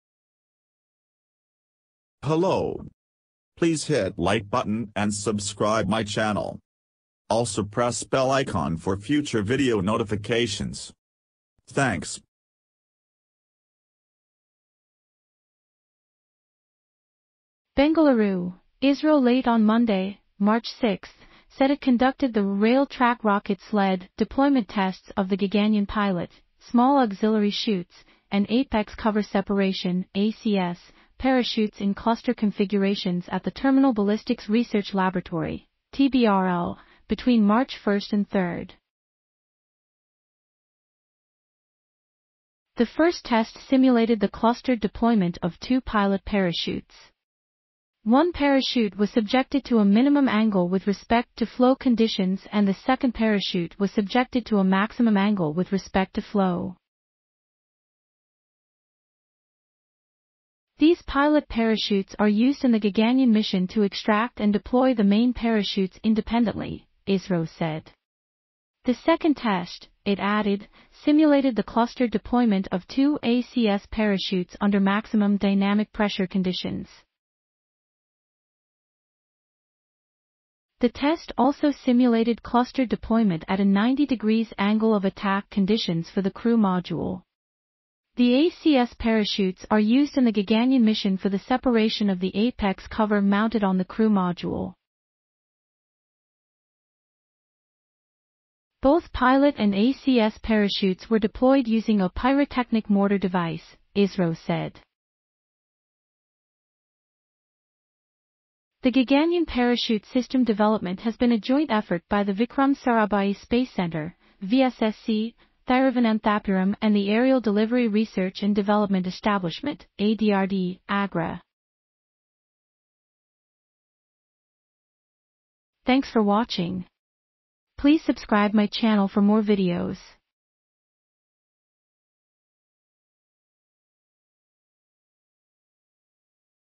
Gaganyaan: Key parachutes deployment tests on rail tracks done
BENGALURU: Isro late on Monday (Mar 6) said it conducted the “Rail Track Rocket Sled” deployment tests of the Gaganyaan pilot (small auxiliary chutes) and apex cover separation (ACS) parachutes in cluster configurations at the Terminal Ballistics Research Laboratory ( TBRL ) between March 1 and 3. “The first test simulated the clustered deployment of two pilot parachutes. One parachute was subjected to a minimum angle with respect to flow conditions and the second parachute was subjected to a maximum angle with respect to flow. These pilot parachutes are used in the Gaganyaan mission to extract and deploy the main...